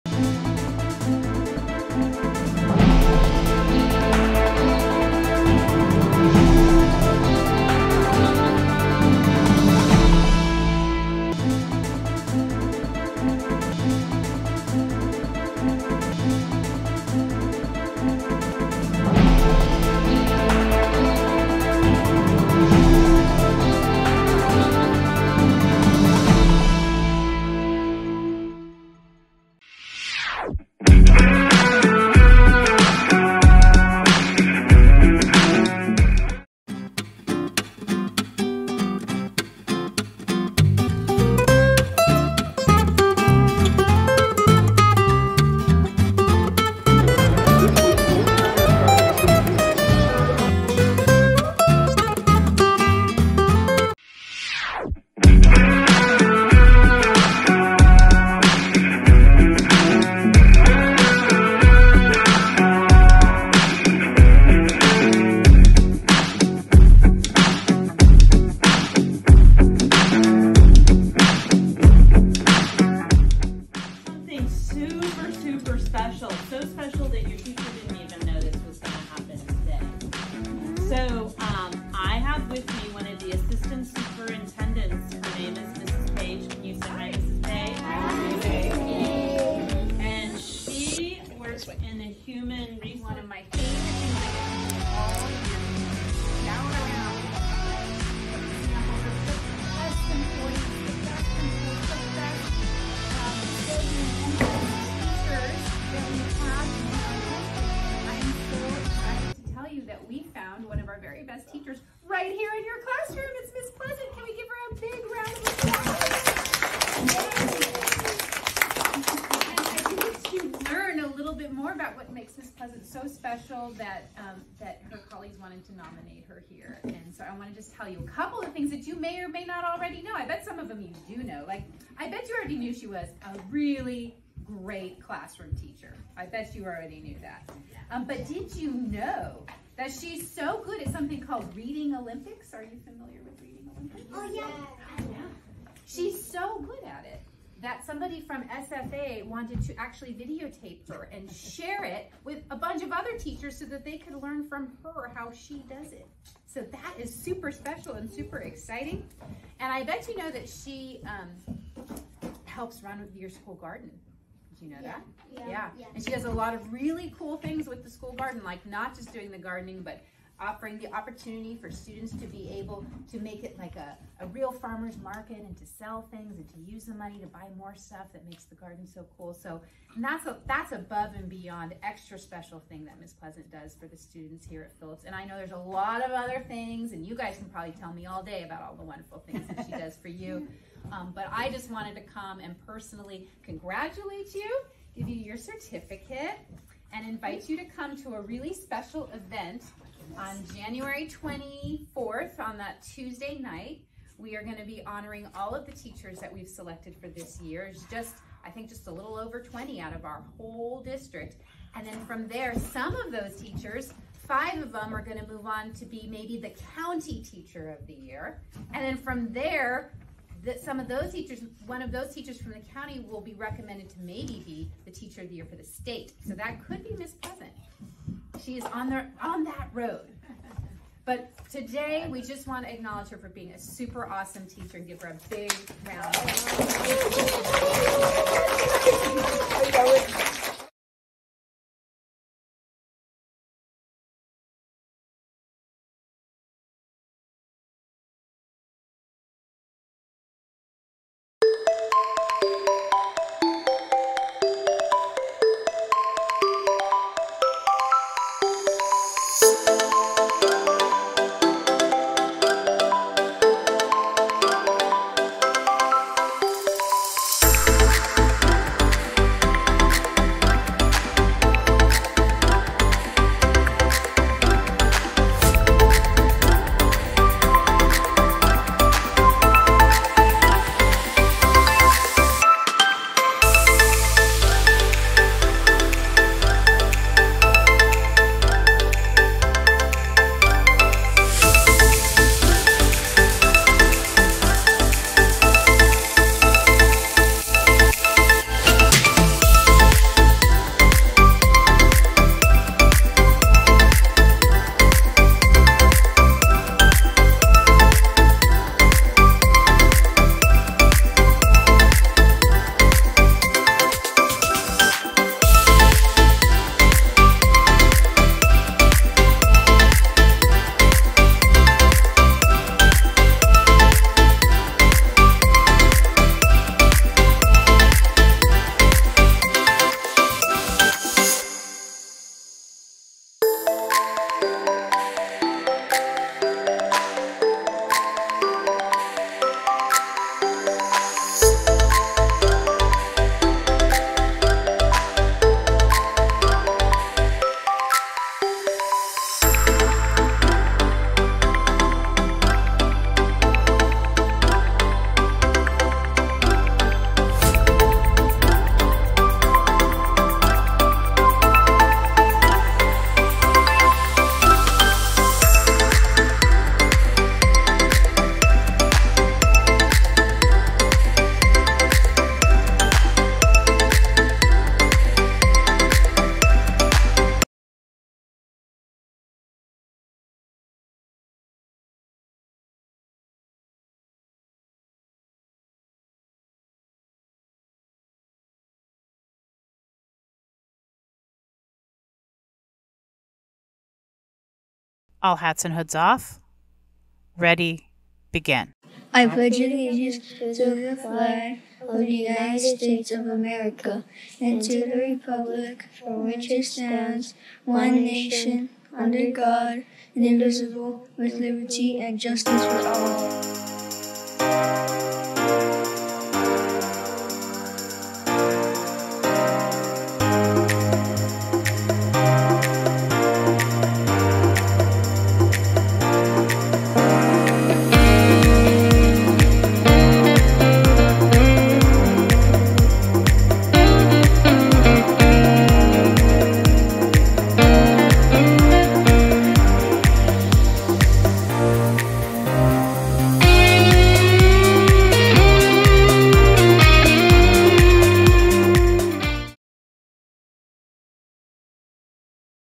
January 17, 2023
Phillips' Leaders finish 2022 strong, and are ready to take on 2023!